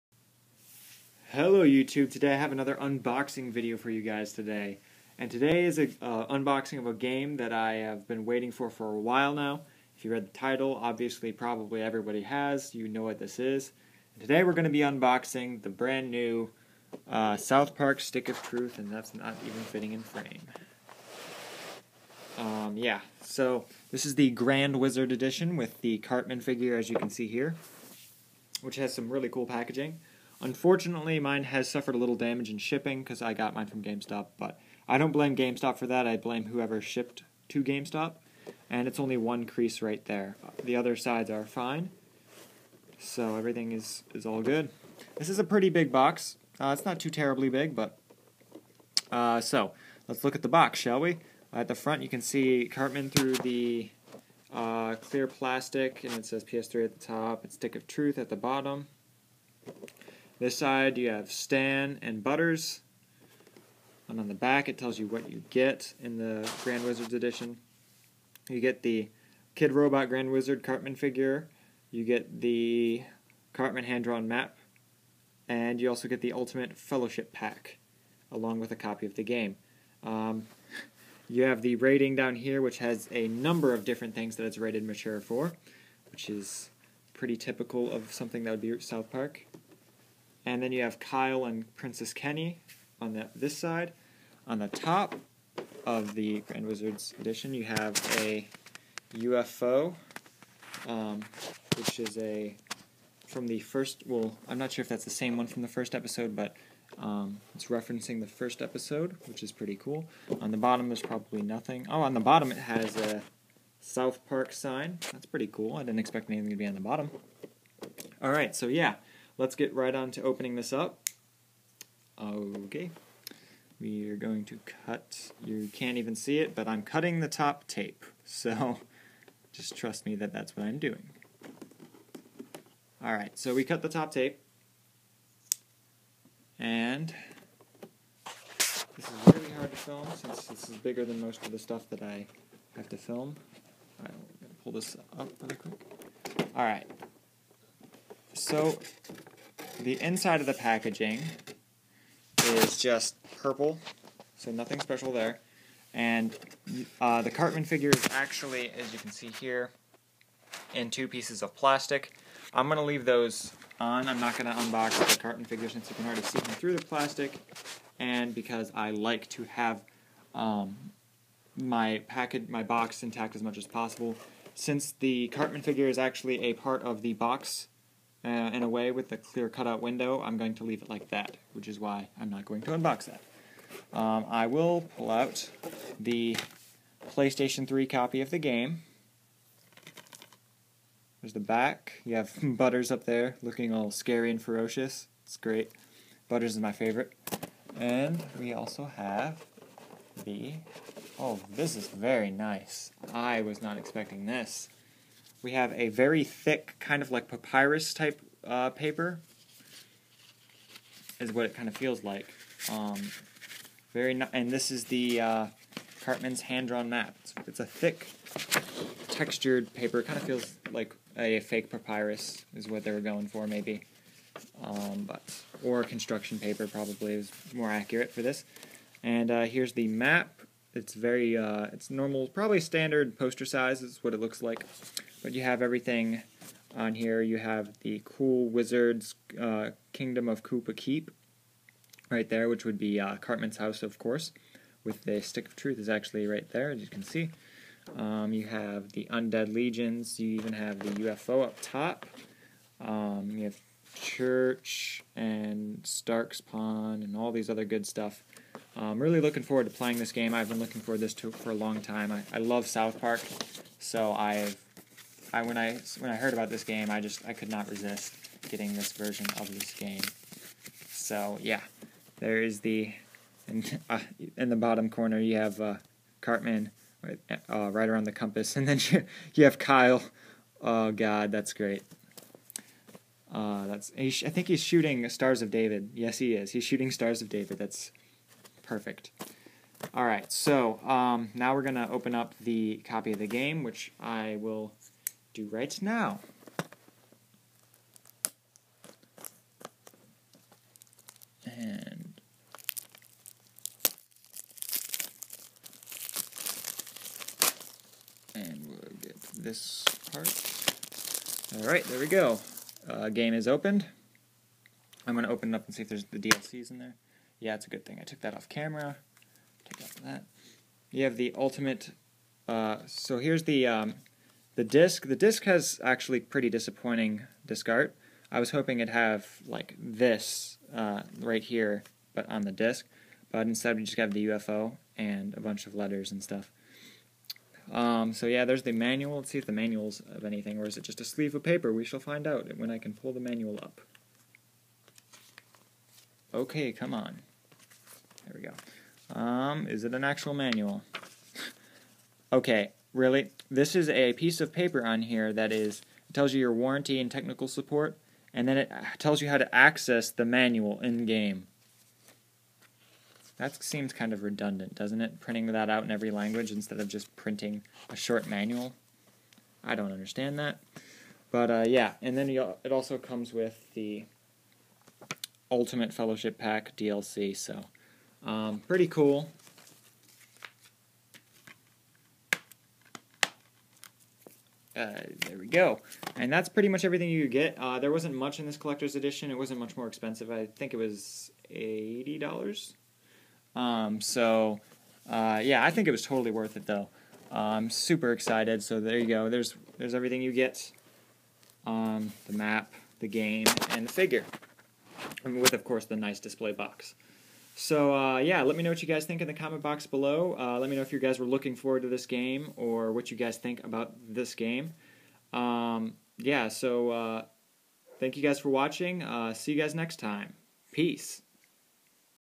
Unboxing South Park The Stick of Truth
Hey everyone I just got my grand wizards edition of south park in today so I thought I would do an unboxing. I hope you all enjoy the video. Let me know what you think of the game so far.